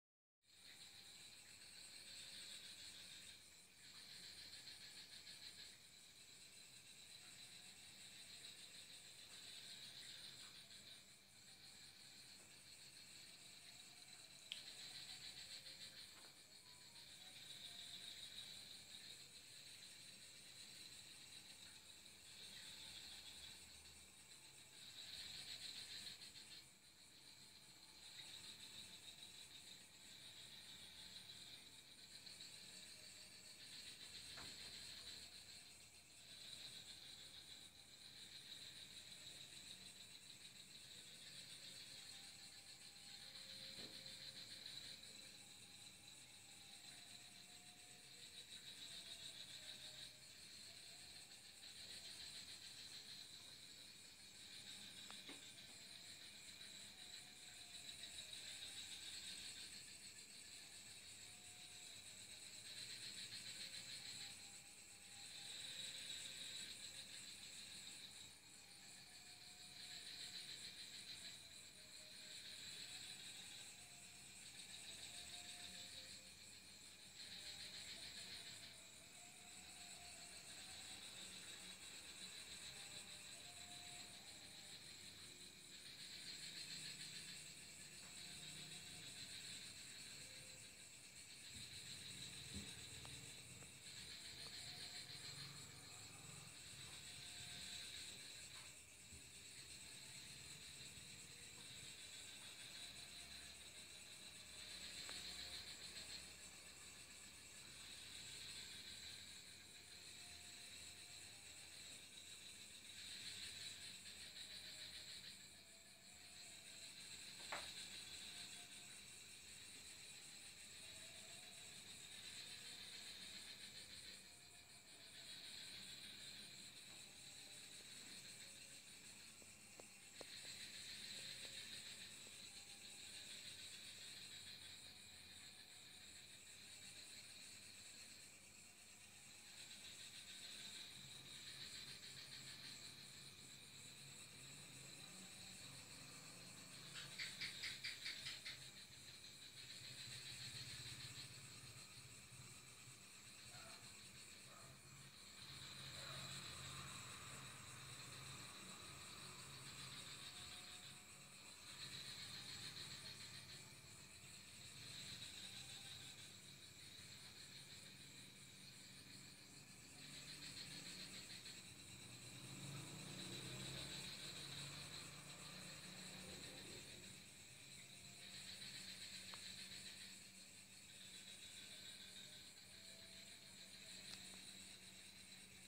Tikus ini tidak takut orang
INFORMASI KAPUAS - Direkam pada hari Jum'at, 16 November 2018 di Jalan Kolonel Sugiono, Kuala Kapuas, Kalimantan Tengah.